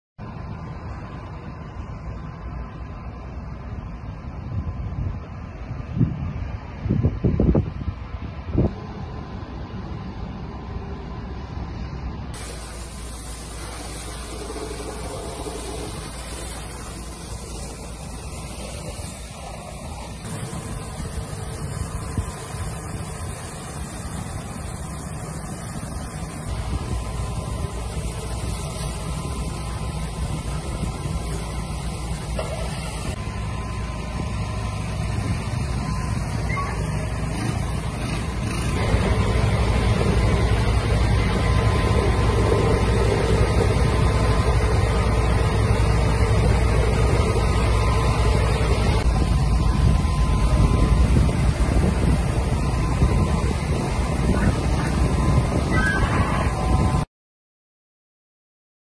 Ancona, vasto incendio nel porto: i vigili del fuoco domano le fiamme
Un vasto incendio è divampato nel porto di Ancona, all'interno del capannone ex Tubimar, dove ci sono state anche delle esplosioni. Nelle immagini l'intervento dei vigili del fuoco, che hanno domato le fiamme. Le operazioni di spegnimento, fanno sapere i pompieri, andranno avanti comunque per tutta la giornata.